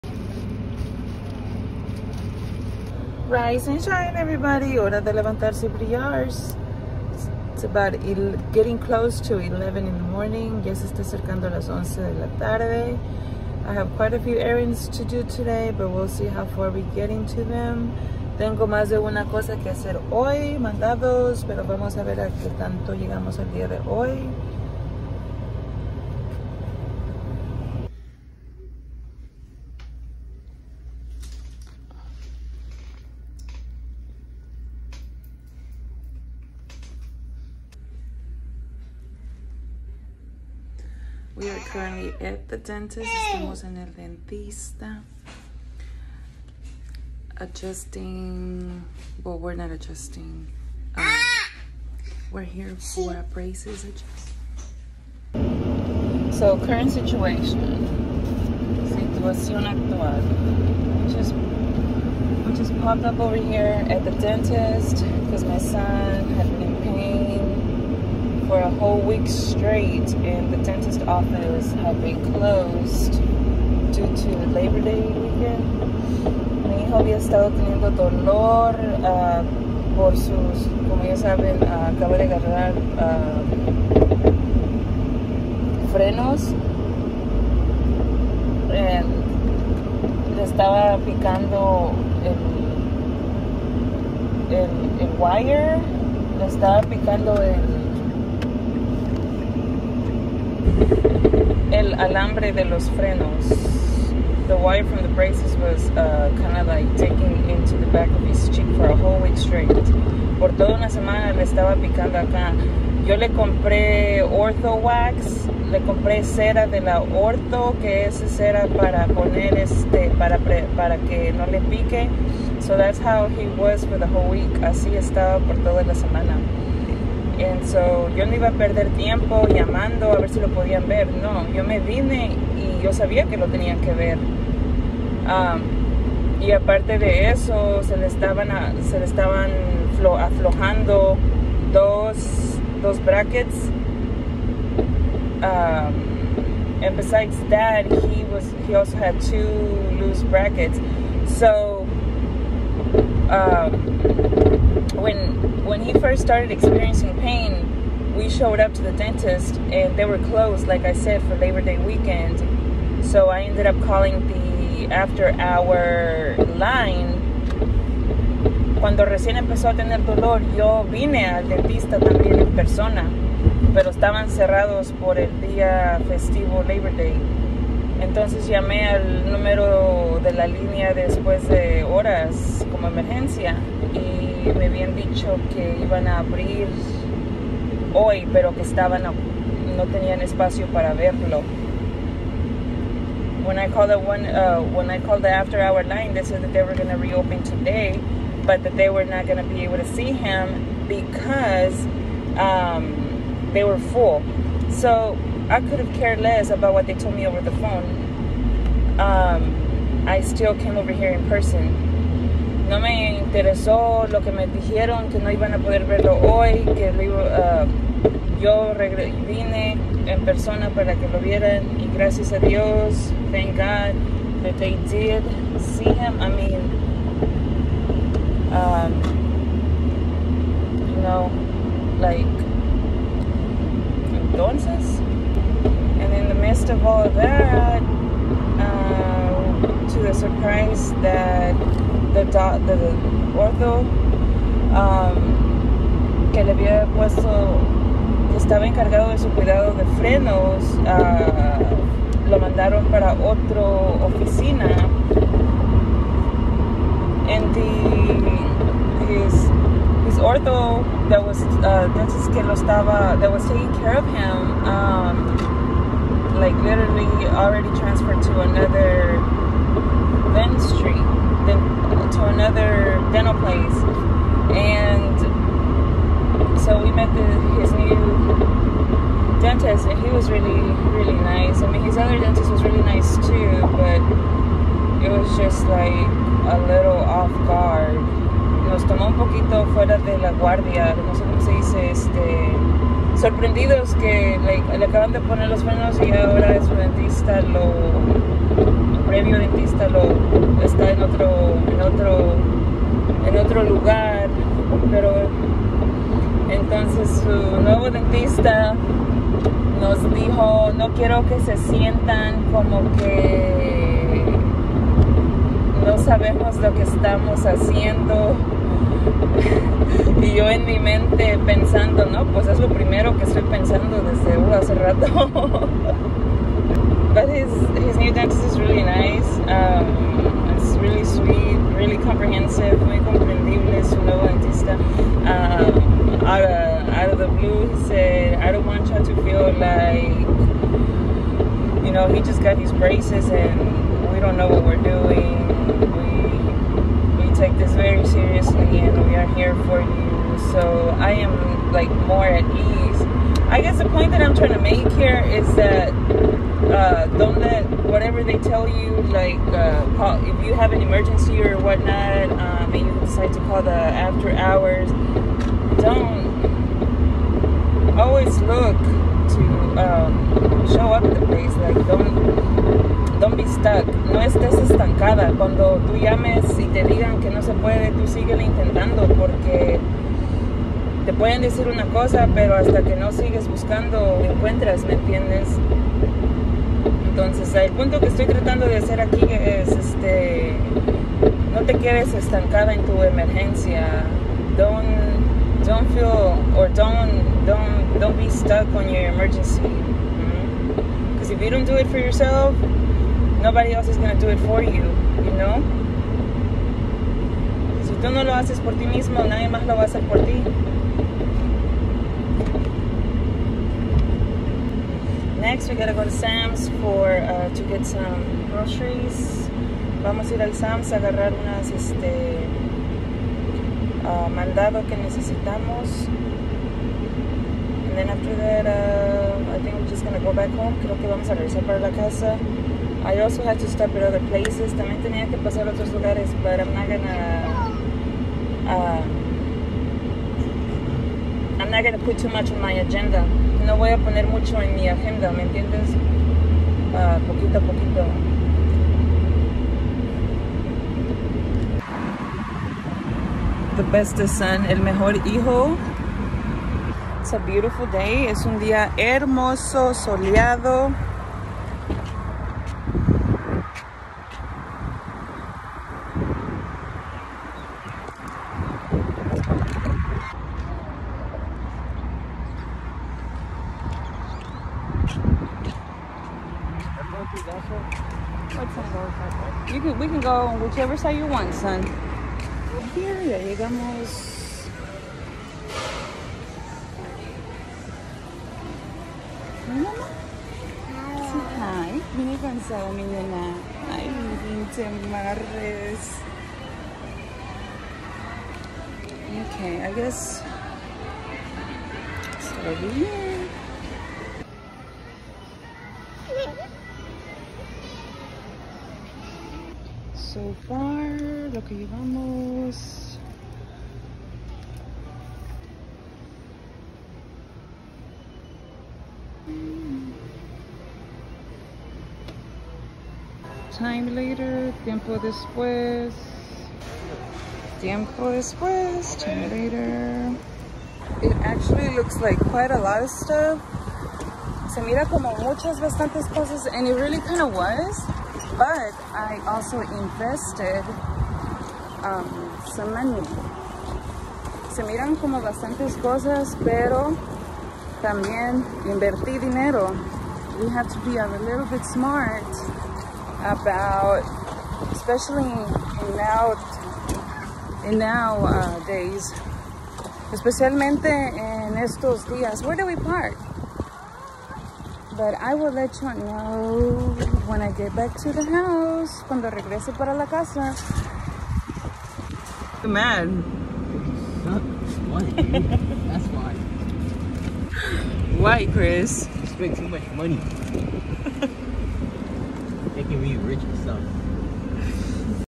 Had to go to the Dentist AGAIN,&Sams Club💞-tube que ir al 🦷 otra vez y Mandado SAMS CLUB
Tuve que ir al 🦷 OTRA VEZ y @

Traer MANDADO @ SAMS CLUB

Feliz&Bendecido Dia

Happy&Blessed